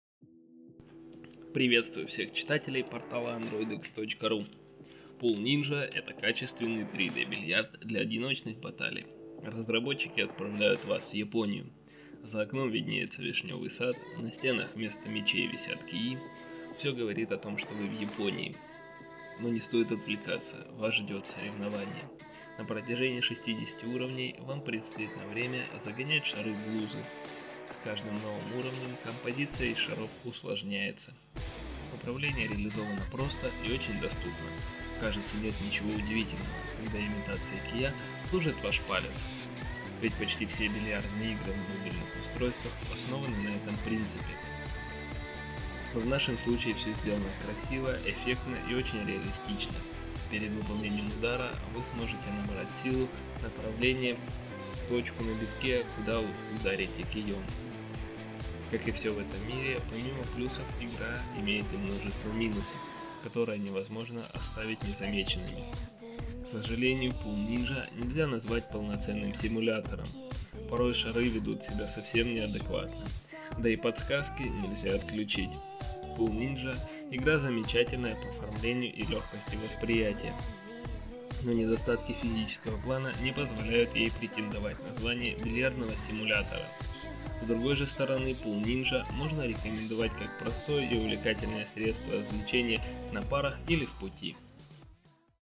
Pool Ninja - японский бильярд
Видео обзор игры для Android - Pool Ninja